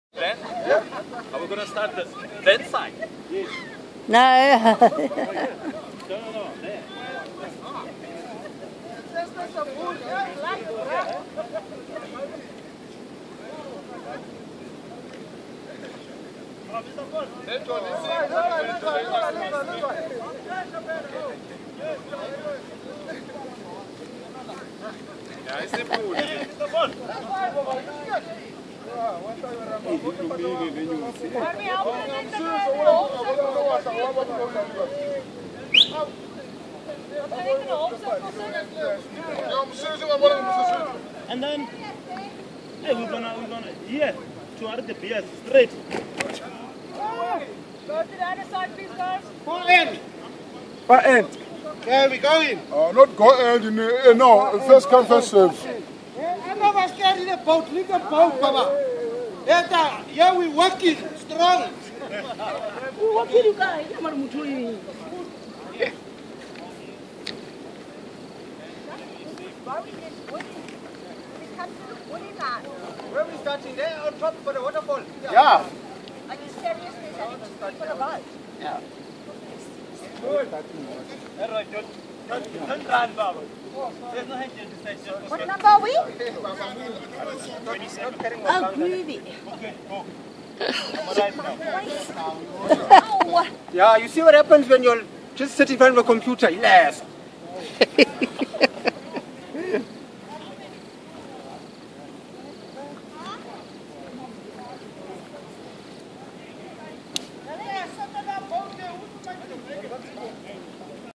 Large corporate group river rafting with Paddle Power
Filled with nervous excitement a large corporate group carry their boats down to the river.